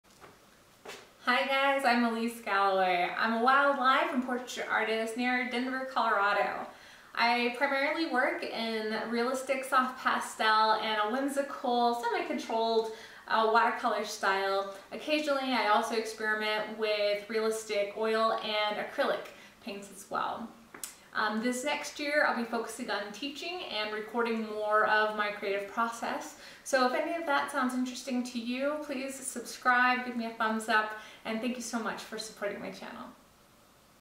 Welcome 2022
Hi guys!
I'm Elyse Galloway. I'm a Wildlife and Portraiture Arist near Denver, CO.
I primarily work in realistic soft pastel and a whimsical, semi-controlled watercolor style. Occasionally, I also experiment with realistic oil and acrylic paints as well. This next year I'll be focusing on teaching and recording more of my creative process.
-Elyse